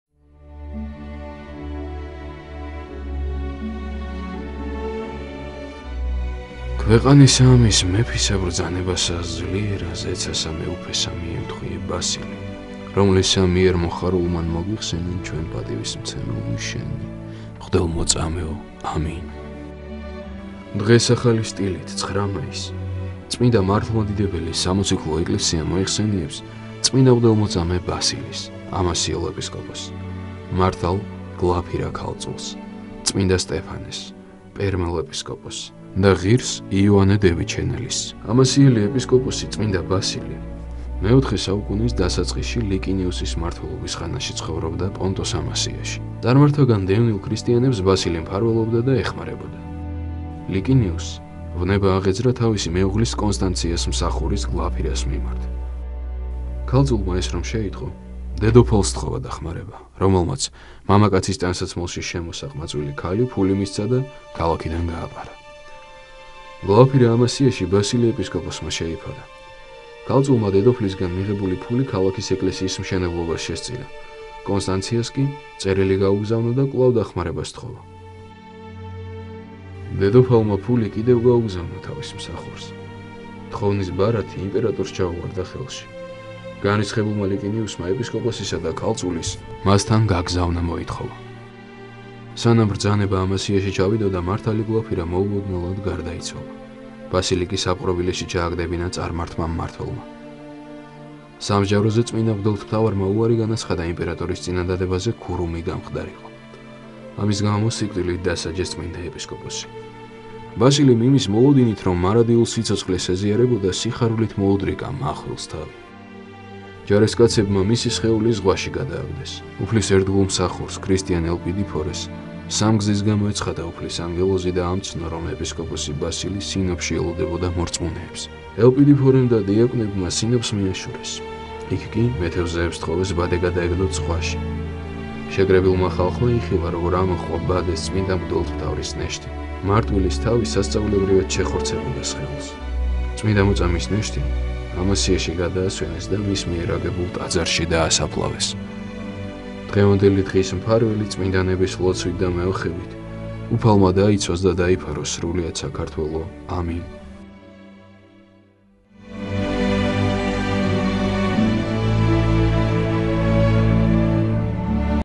საეკლესიო კალენდარი - 9 მაისი
saeklesio kalendari - 9 maisi ; მღვდელმოწამისა ბასილისა, ამასიელი ეპისკოპოსისა (დაახ. 322); მართლისა გლაფირა ქალწულისა (322); წმ. სტეფანესი, პერმელი ეპისკოპოსისა (1396); ღირსისა იოანიკე დევიჩენელისა (XIII) (სერბ.).